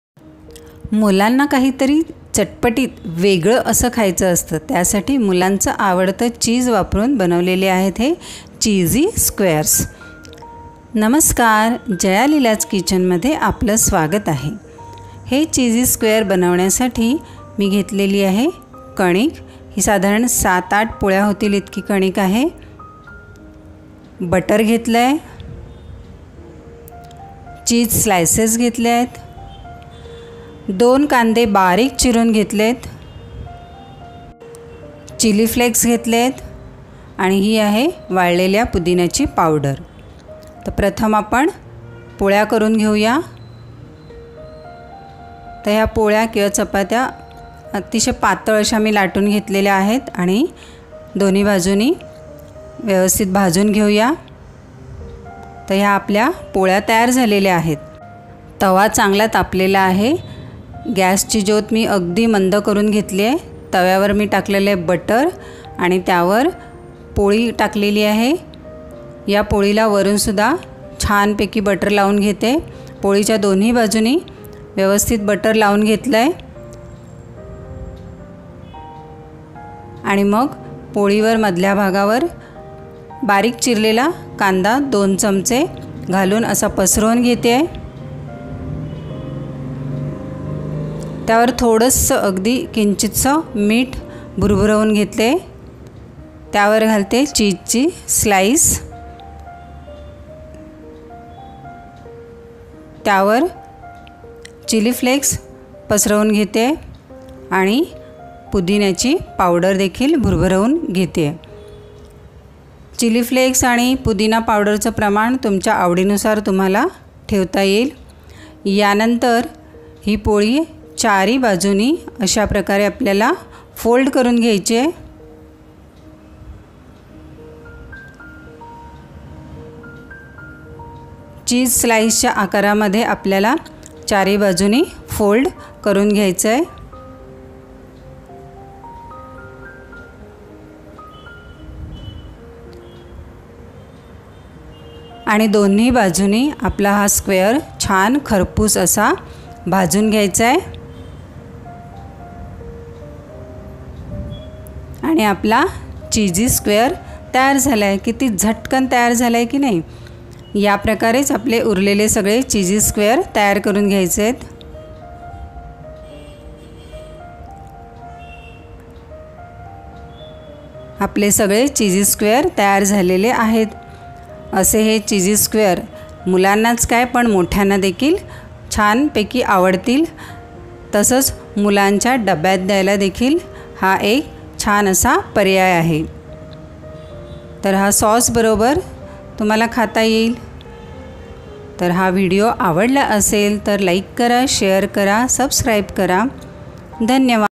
मिंट ओनिअन चीजी स्क्वेअर्स | झटपट चीज पराठा | how to make cheesey squares | easy cheese paratha
साहित्य

भिजवलेली कणिक           ६ते७ पोळ्यांना (चपात्यांना)
                                      पुरेल इतकी
चीज स्लाईसेस                  ६ते७
कांदे                                २ बारीक चिरून
चिली फ्लेक्स                     चवीनुसार
पुदिना पावडर                    चवीनुसार
मीठ                                  चवीनुसार
बटर                                  ३ Tbsp

ताज्या पोळ्यां ऐवजी शिल्लक राहिलेल्या पोळ्यापासून देखील चीजी स्क्वेअर्स तयार करता येतील.

Ingredients

wheat dough                enough for 6to7    chapatis
cheese slices               6to7
onions                           2 fine chopped
chilli flakes                   as per taste
mint powder                 as per taste